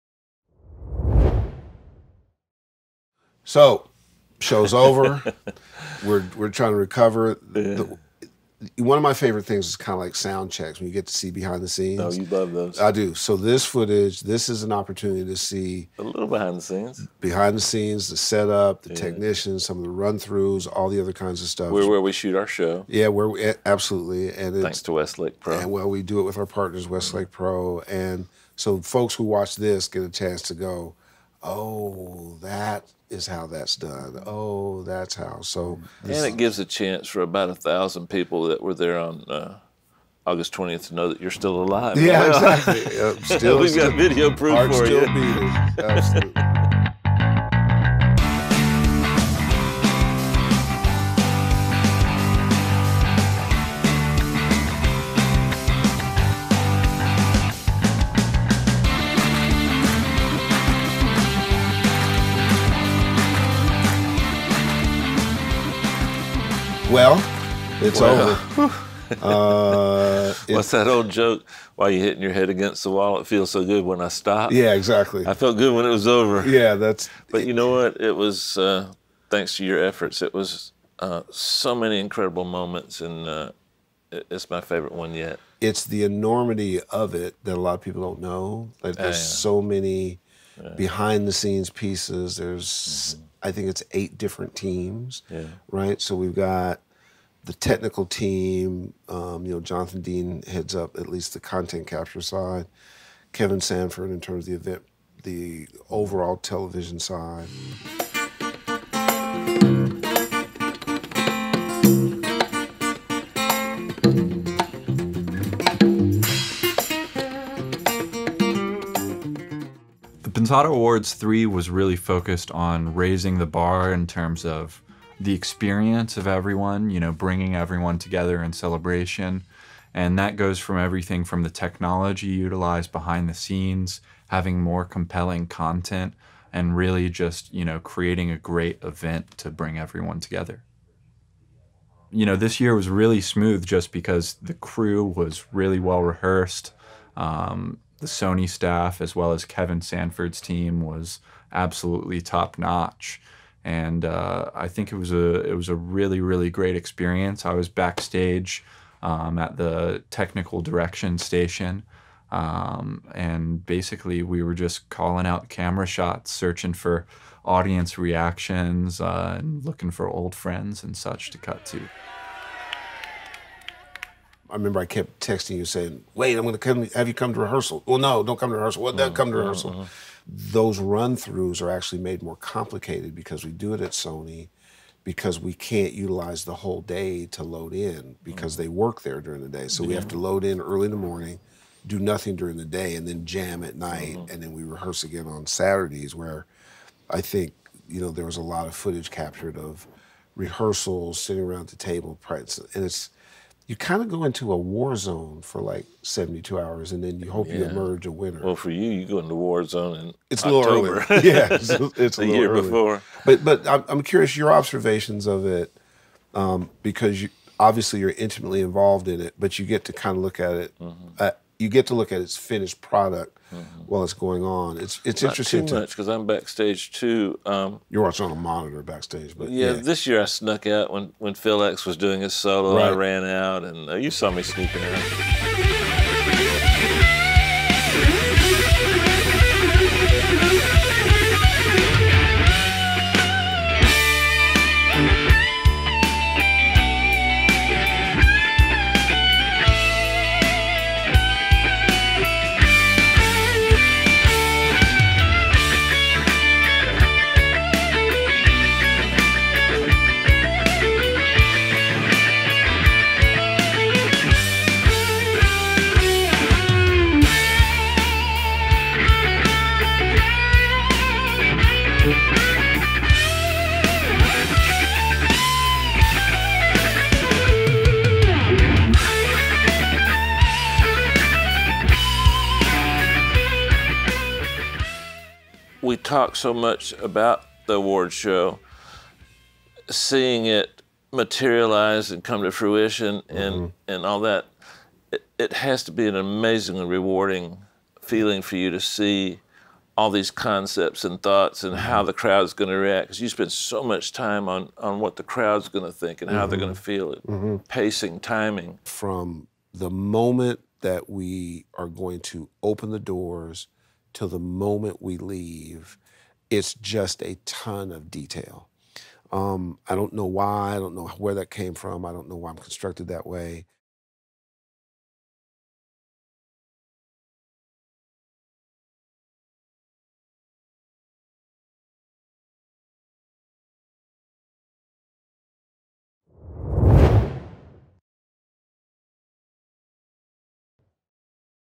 Behind the Scenes at Pensado Awards 3 - Part 1 | Westlake Pro
Take a look behind the scenes at the Pensado Awards 3 with this exclusive mini-documentary produced by Westlake Pro.

In part 1 of this video, we hear from Dave, Herb, and Westlake Pro CTO and producer of Pensado's Place Jonathan Deans as they give us a detailed behind the scenes look at PA3.